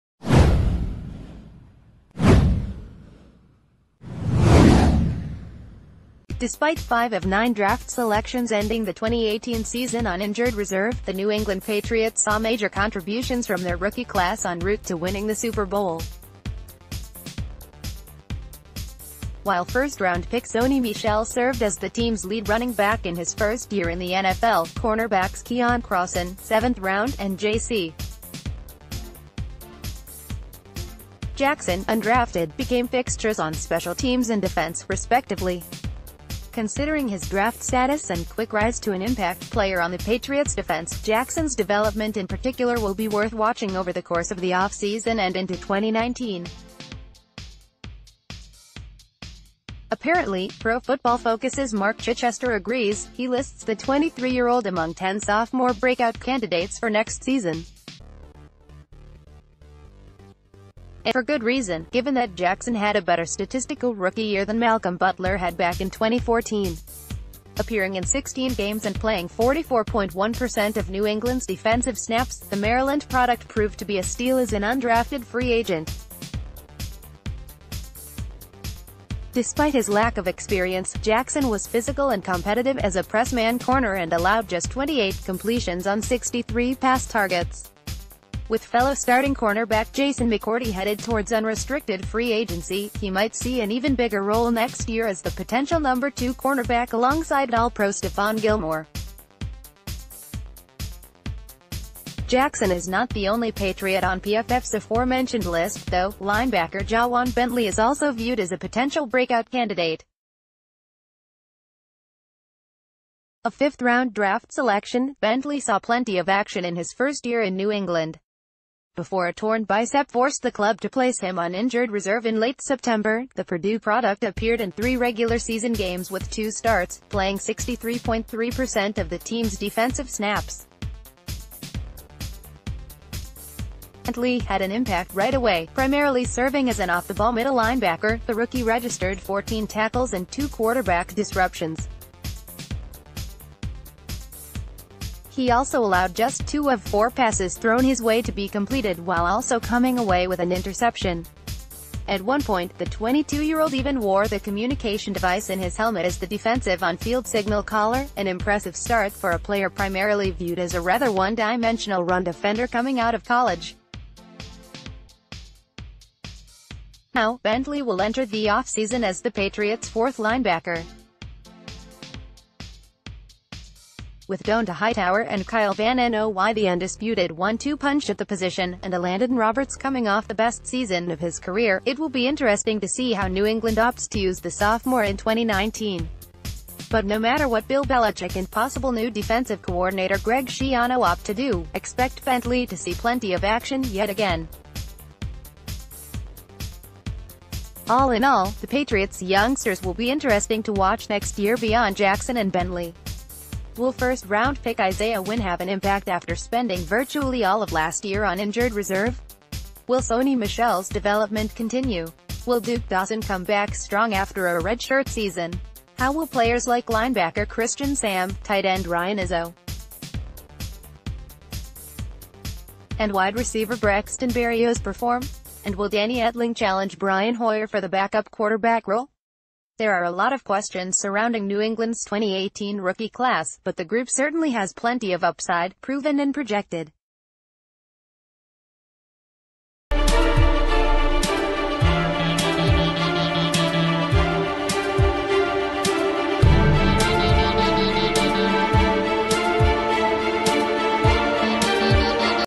2 Patriots among Pro Football Focus’ sophomores bound to break out in 2019
2 Patriots among Pro Football Focus’ sophomores bound to break out in 2019
2 Patriots among Pro Football Focus’ sophomores bound to break out in 2019

Despite five of nine draft selections ending the 2018 season on injured reserve, the New England Patriots saw major contributions from their rookie class en route to winning the Super Bowl. While first-round pick Sony Michel served as the team’s lead running back in his first year in the NFL, cornerbacks Keion Crossen (seventh round) and J.C. Jackson (undrafted) became fixtures on special teams and defense, respectively. Considering his draft status and quick rise to an impact-player on the Patr...